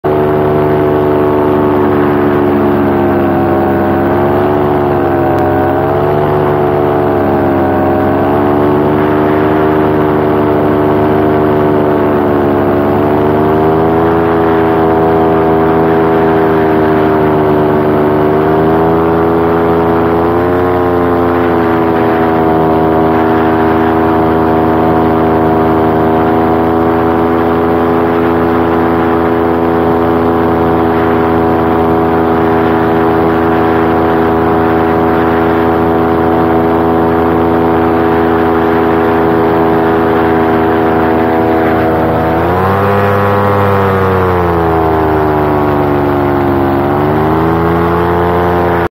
low and fast, lowpass over the seashore in 2024
źródło: PRO VID 2024

how to fly super fast a paramotor